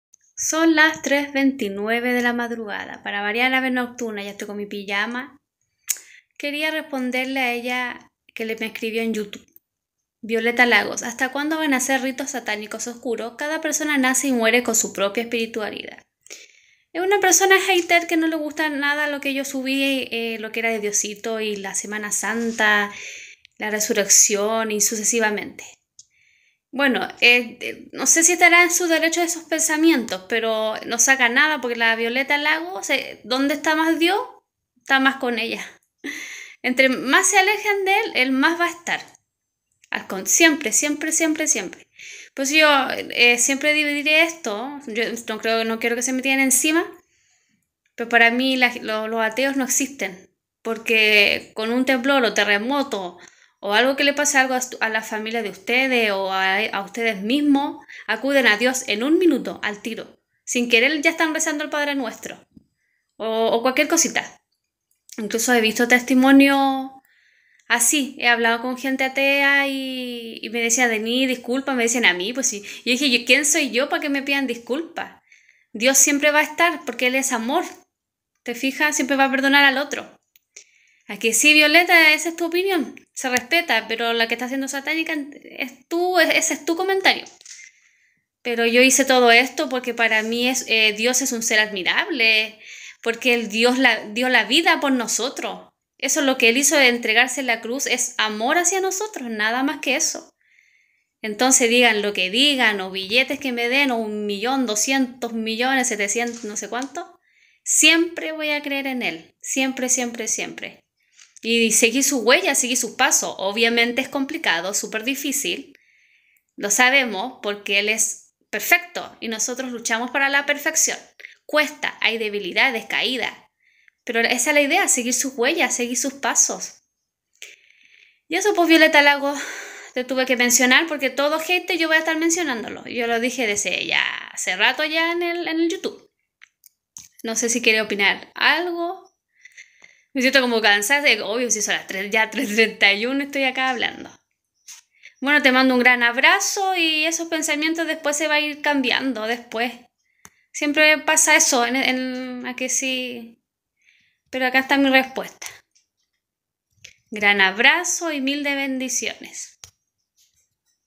Aca tu respuesta Violeta Lagos, todo hater será mencionado!!
Respondiendo a unos de los cuantos hater que tengo en mi youtube siempre serán mencionados, los que hagan mal comentarios, o ofensas hacia a mi. Aca su respuesta